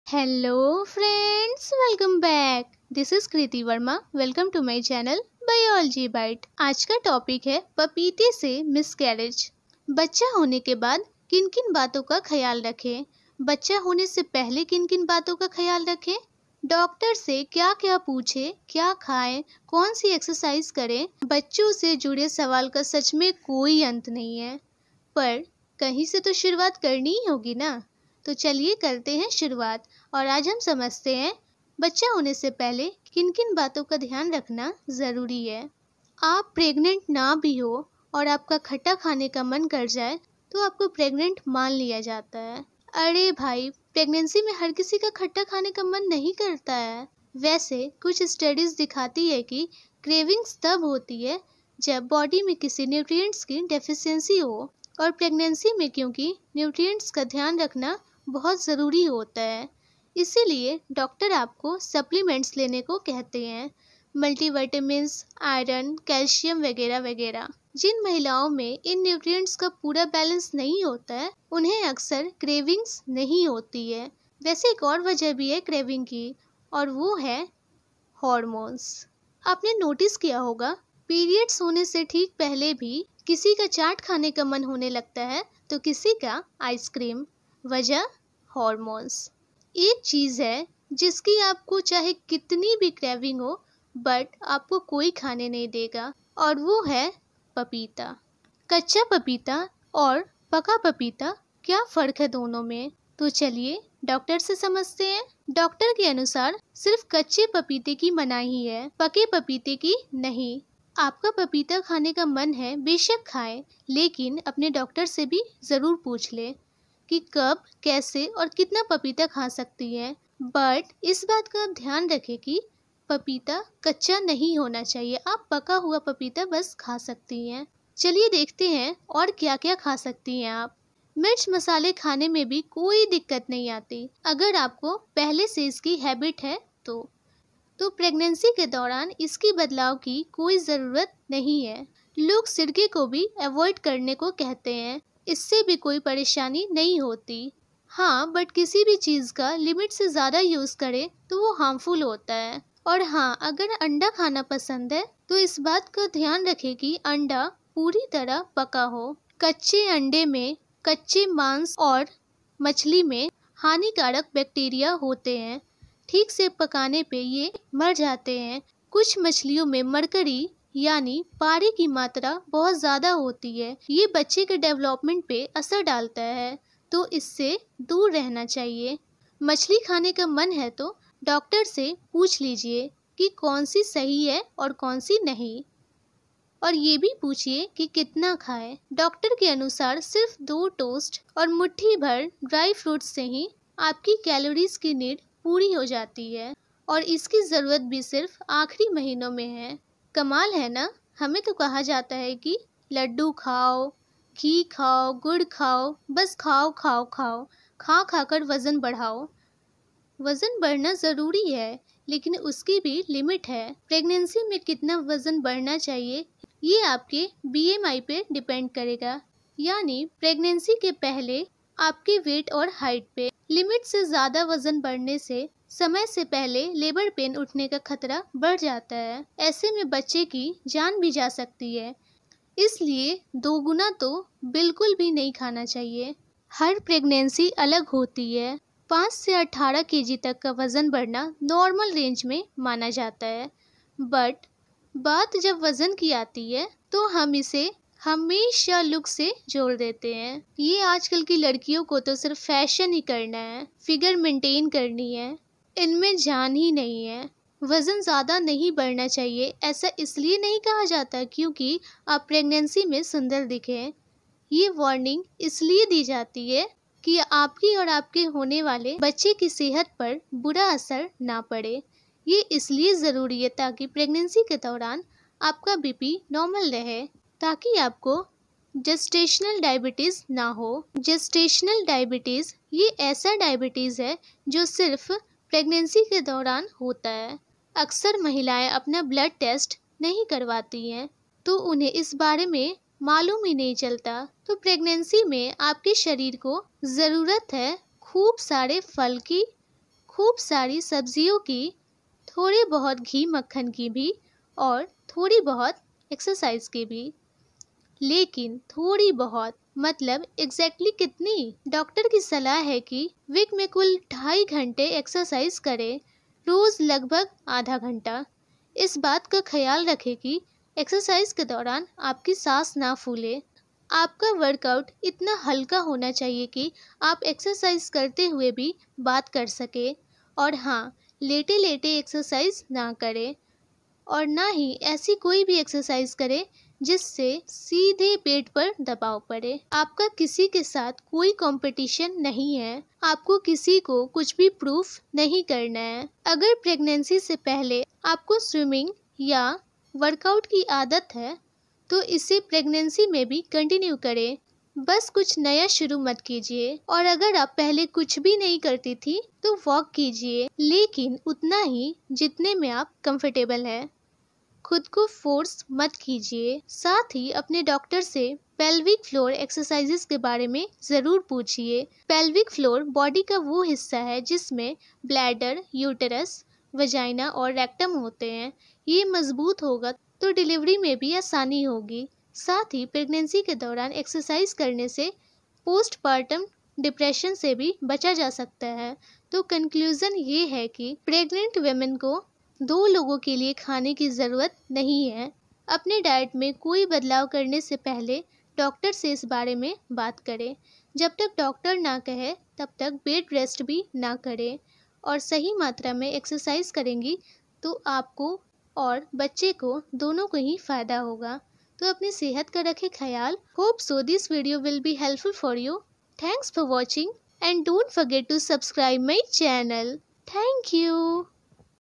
Is papaya harmful during pregnancy? ।। Health tips for pregnant women ।। Biology Byte By Kriti
Hello everyone ❤️ Myself Kriti Verma ✨
In this lecture vdo we discussed about health tips for pregnant women

पपीते से Miscarriage?

Vdo ko end tk dekhe and apne dosto ke saath share kare ❤️

Health tips for pregnant women
Always stay healthy and fit

Biology Byte By Kriti ✨
Show me ur love 💕